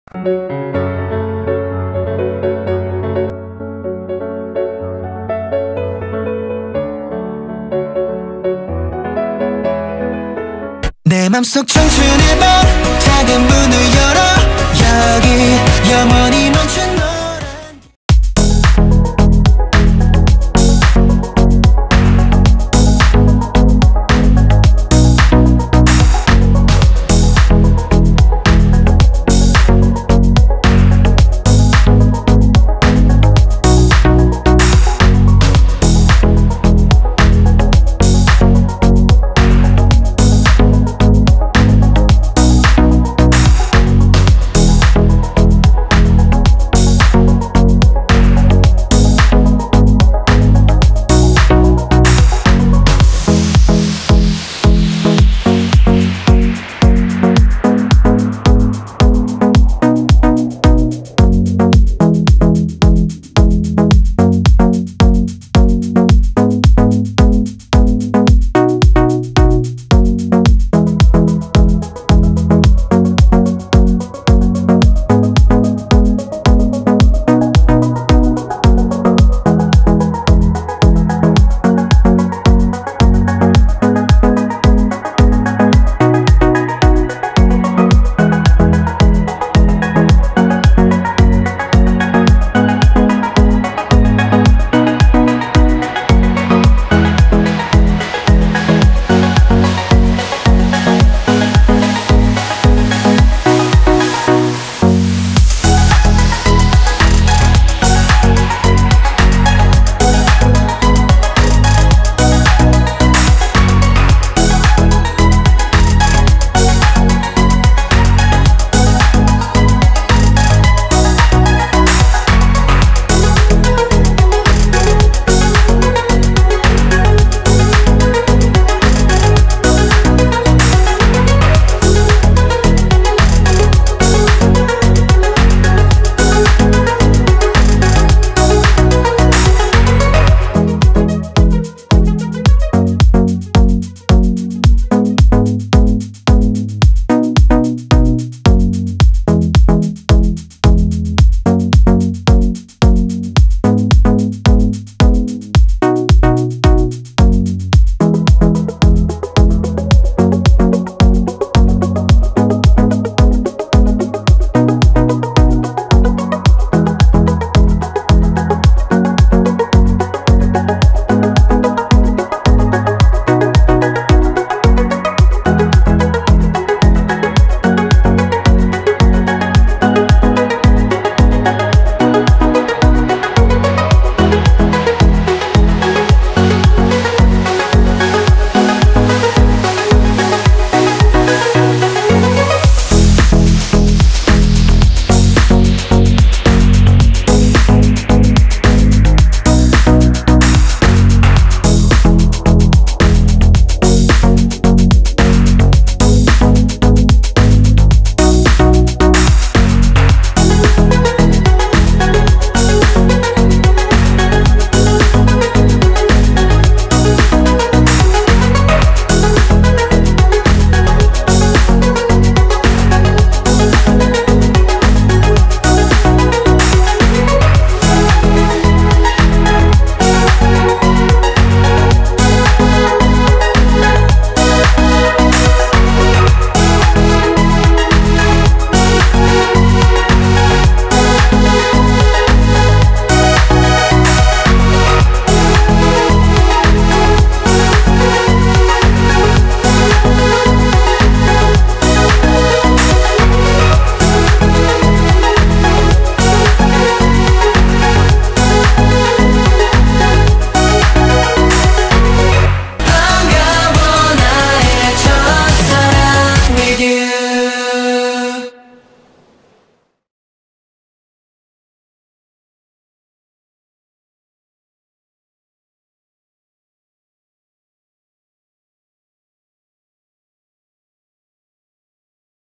The Wind (더윈드) MEMBERS PROFILE & FACTS [GET TO KNOW K-POP BOY GROUP]
💨 The Wind (더윈드) MEMBERS PROFILE & FACTS 💨

Welcome to the comprehensive guide on the members of The Wind!In this video, you'll learn each member of K-pop group "The Wind", sharing detailed profiles, and intriguing facts. Whether you're new to the fandom (WHIZ) or a long-time supporter, this video will help you get to know The Wind like never before.

Intro (00:00)
The Wind (00:17)
Kim Hee Soo (00:34)
Thanatorn (01:06)
Choi Han Bin (01:38)
Park Hayuchan (02:10)
An Chan Won (02:42)
Jang Hyoun Joon (03:14)
Former Member (03:46)

Dive into the world of The Wind with us and discover what makes each member unique.